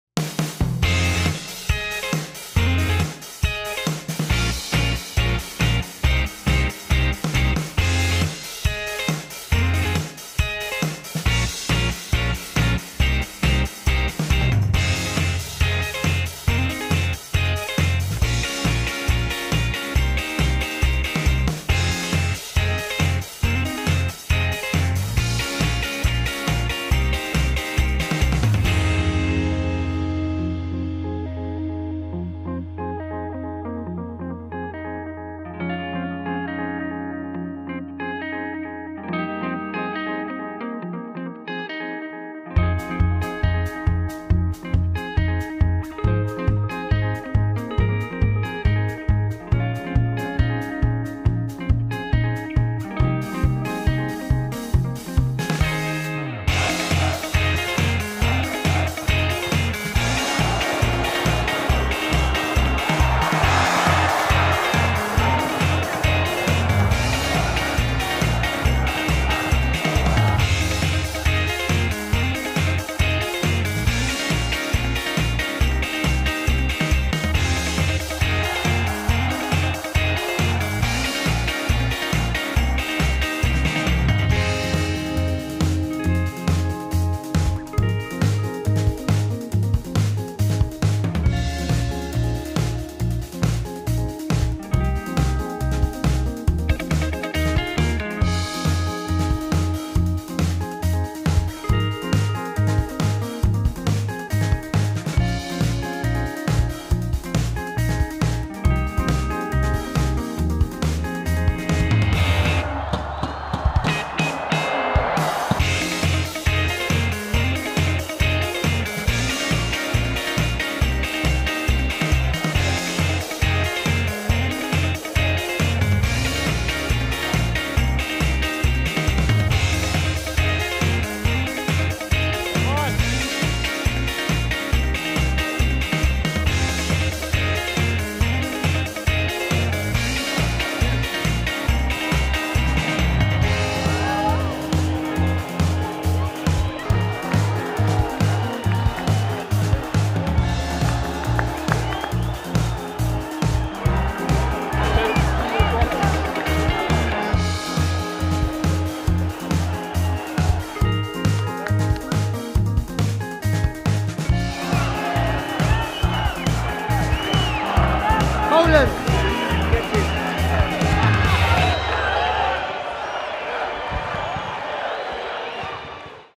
Pitchside Highlights - Kazakhstan U21 0-1 England U21 (Euro-17 U21Q) | Matchday
Go behind the scenes at the Central Stadium as FATV's cameras capture all of the action of the European Championship qualifier between Kazakhstan U21 and England U21.

An early goal from Demarai Gray (Leicester City) maintained England U21s unbeaten record as they sealed their spot for next summers tournament in Poland.